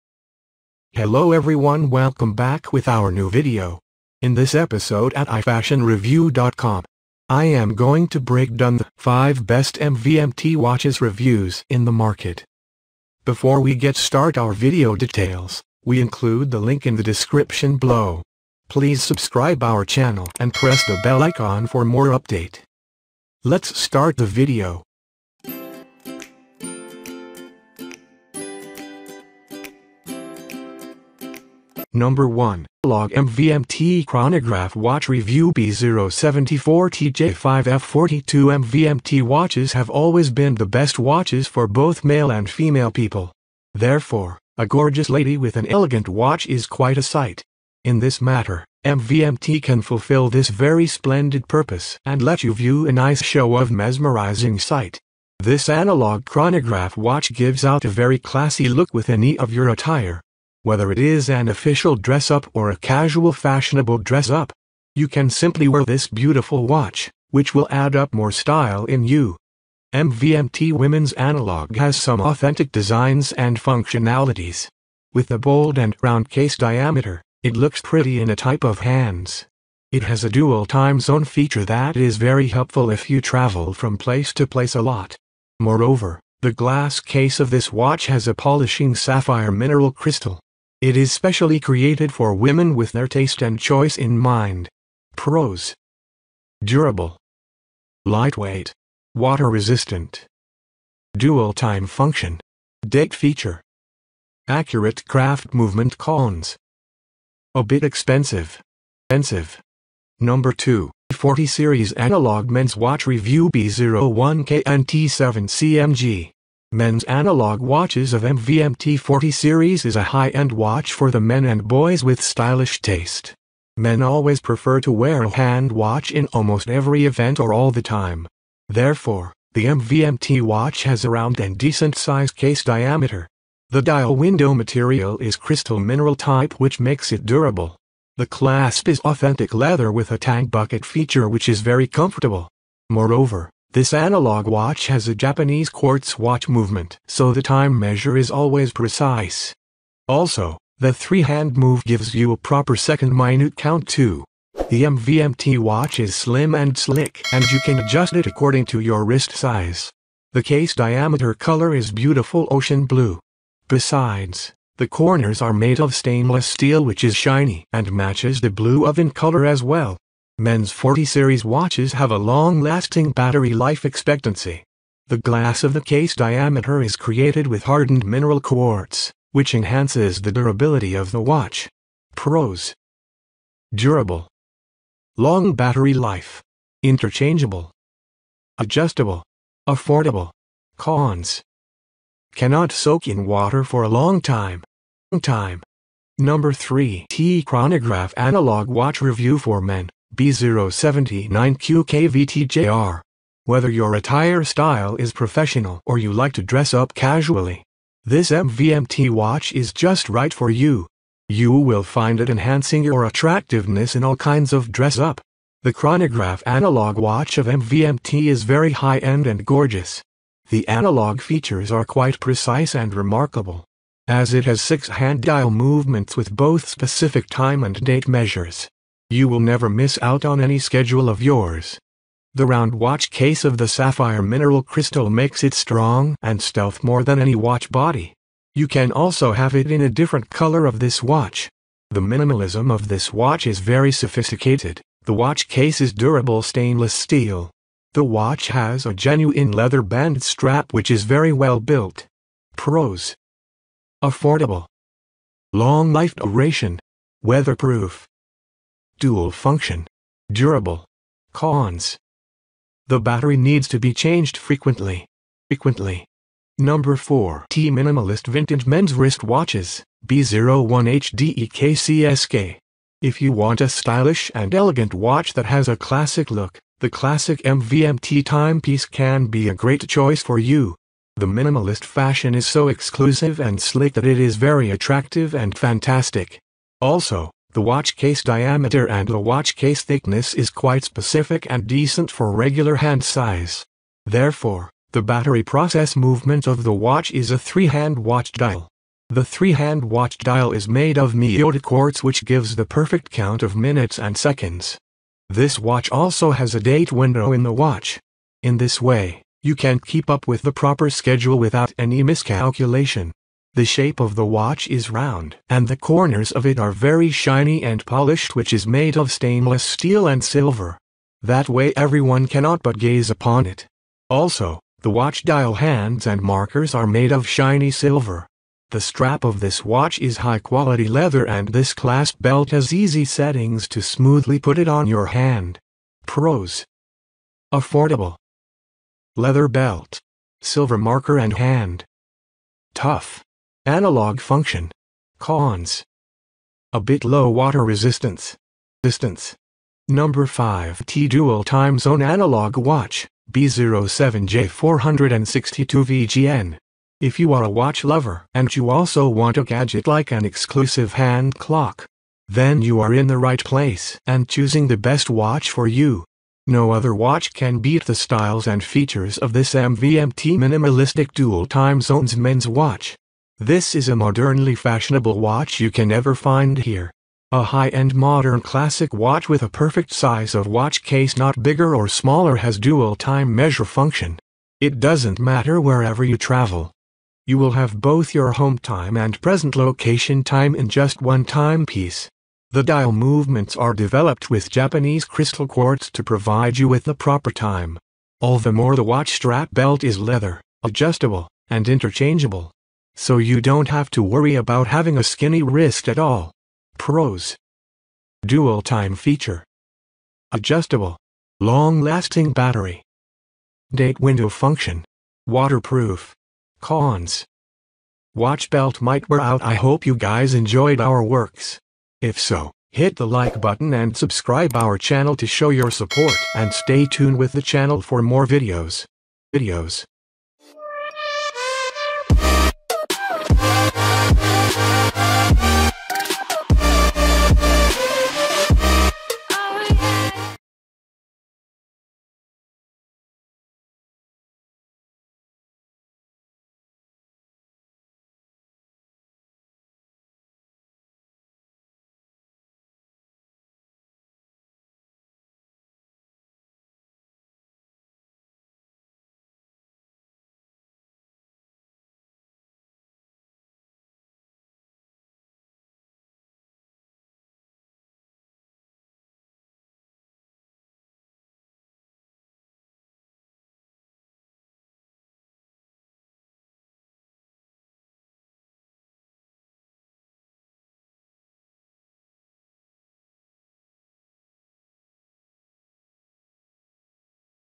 Best MVMT Watches Review
MVMT watches reviews are for you to know about the best watches of the new trending MVMT watch brand.

This is a newly built watch brand in the USA which is offering both exclusively high standard and high functionality watches. Also, you will find very attentive designs and styles in the watches of this brand.

Moreover, even analog watches possess high-end functions and features. For example, almost all watches are very durable and stylish. As the watch body is created with crystal and stainless steel.

Further, every single watch has preferable features of adjustable size, interchangeable or replaceable belt straps.

Also, the watch has dual time zone features, date dial window features, and so on. Also, it has a Japanese quartz precise time movement and the chronograph 6 hands in the watch.

Thus, here are the MVMT watches reviews. So you can select the best one according to your choice and taste.